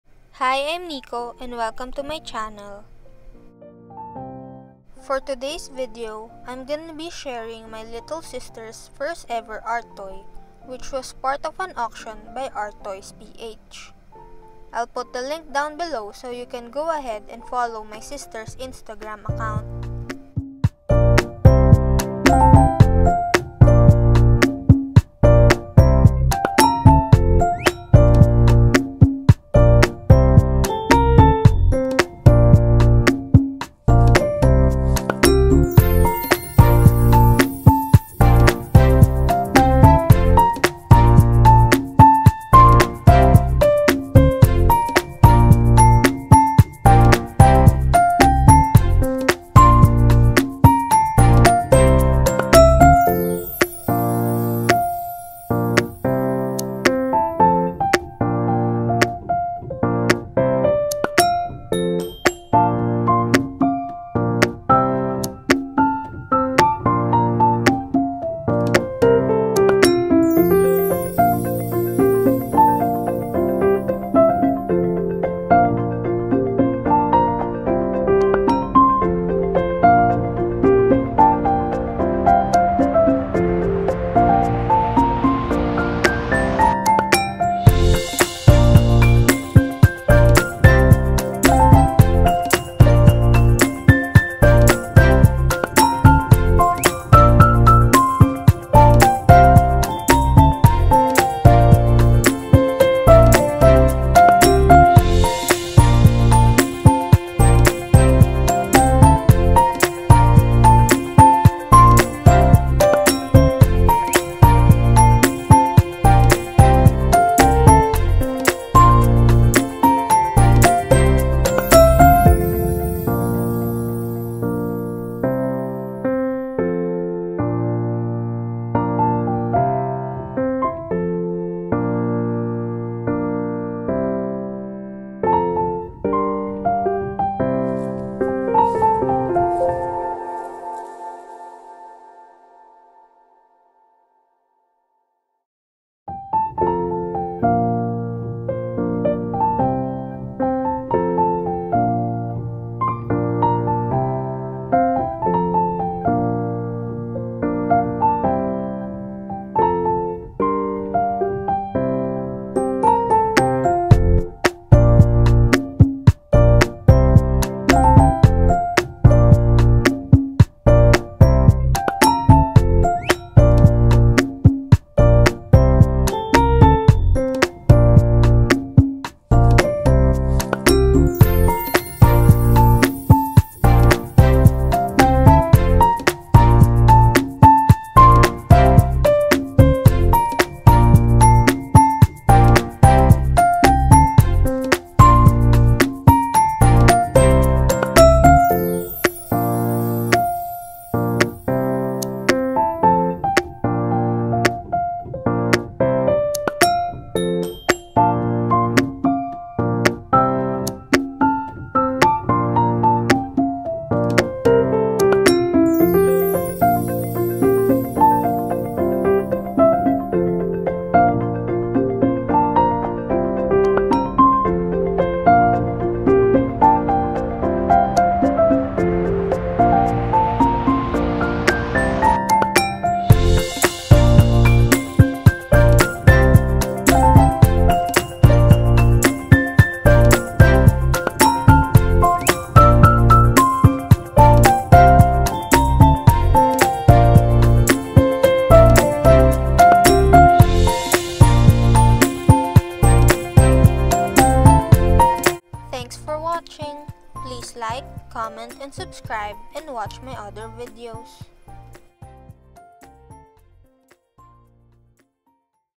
Custom Painted ART TOY by my Little Sister! | Her FIRST Designer Art toy!
Custom Painted Art Toy for an Auction held by Arttoysph. My little sister painted her FIRST EVER Art Toy!